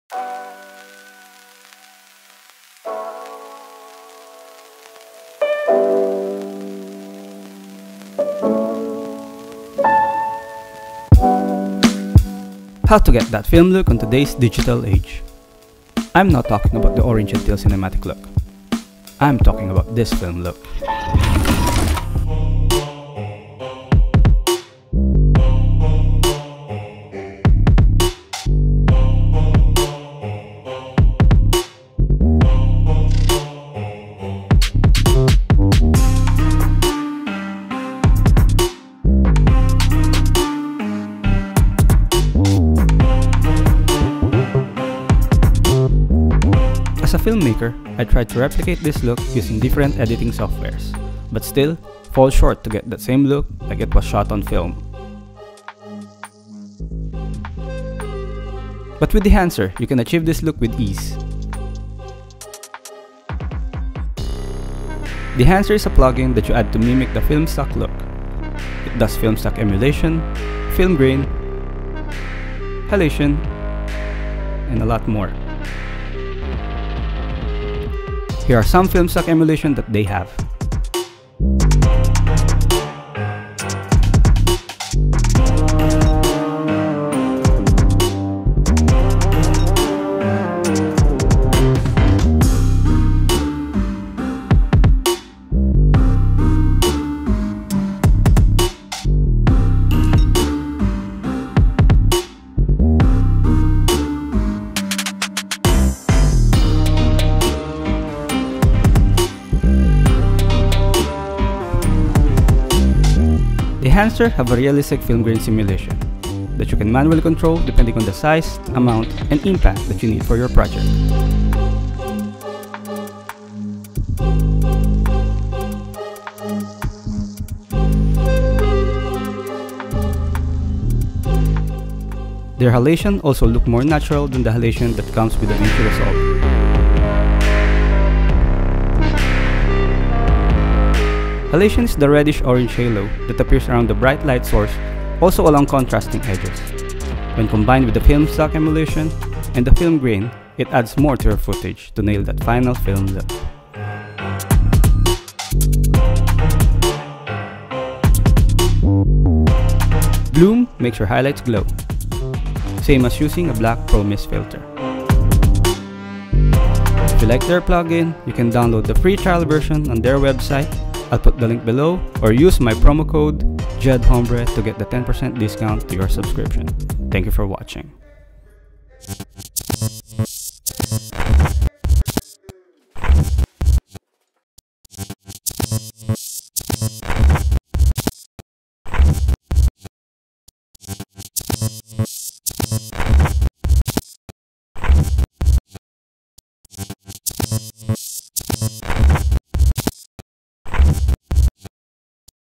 Getting that "FILM LOOK" with Film Emulation - (DEHANCER PLUG-IN)
I grew up watching films even before the digital age. As much as I love the movies today, I still have that love for the old FILM LOOK. I have tried using many methods to replicate that look by watching on youtube but the best method is using the @Dehancer Film Emulation Plug-in.

If you love this kind of look I suggest you try their plug-in.